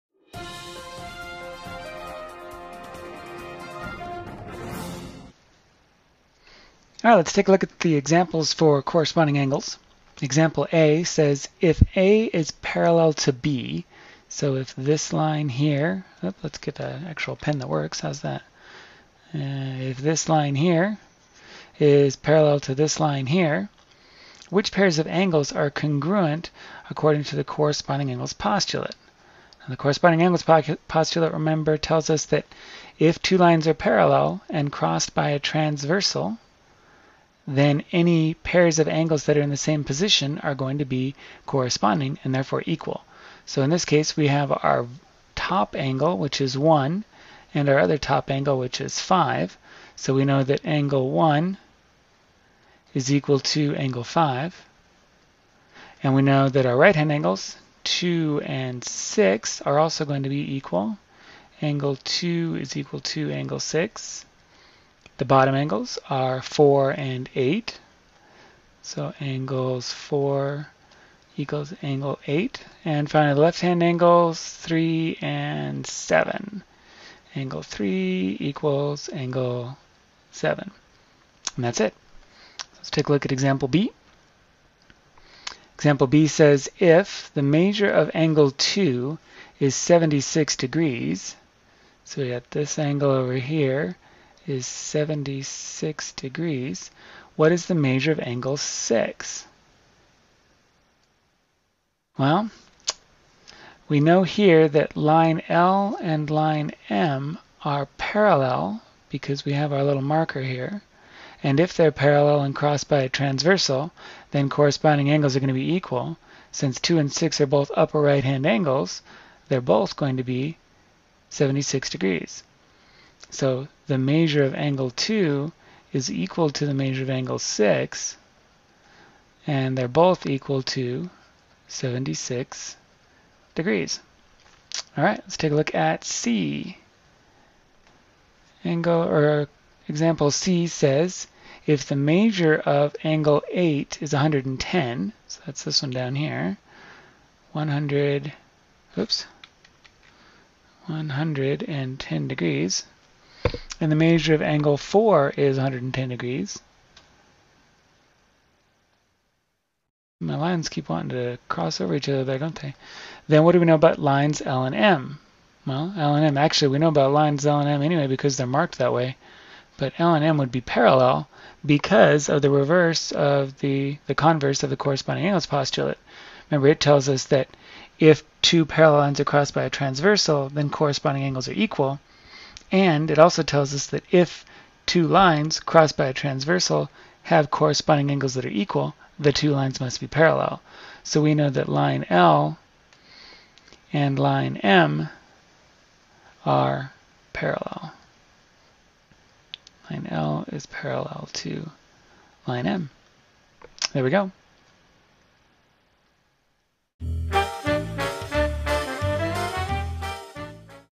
Corresponding Angles: Examples (Basic Geometry Concepts)
Here you'll learn what corresponding angles are and what relationship they have with parallel lines.

This video provides the student with a walkthrough of one or more examples from the concept "Corresponding Angles".

This is part of CK-12’s Basic Geometry: Parallel and Perpendicular Lines.